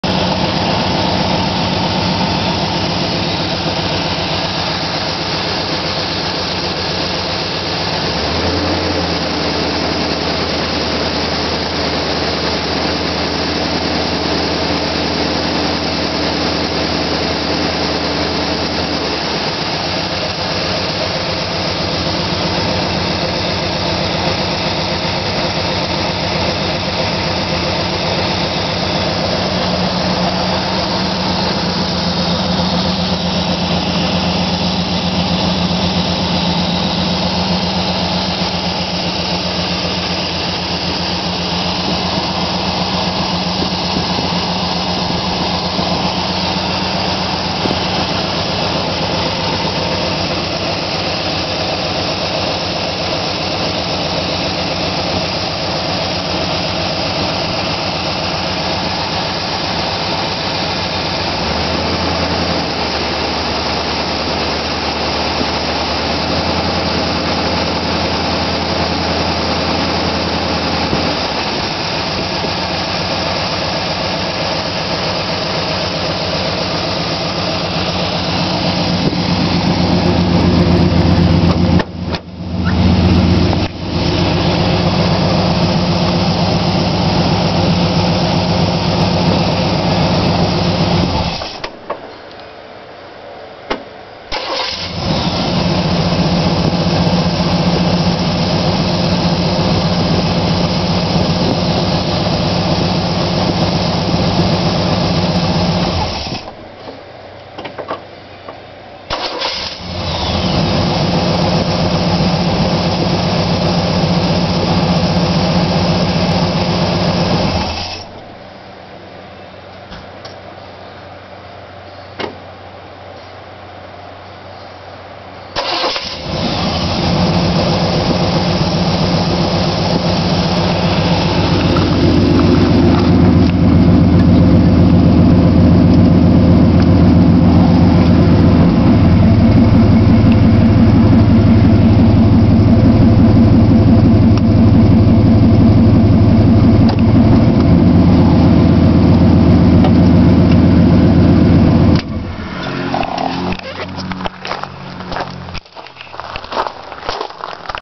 Chevy 350 (first run)
JUDGE
First Running of Chevy 350 in a '57 210
Complete engine swap in 3 days. Last Thursday found crack in block..
By the following Friday engine/ parts delivered and installed.
Primed the motor, put power to the coil and first shot..
the results as you see..
It's all you Bri..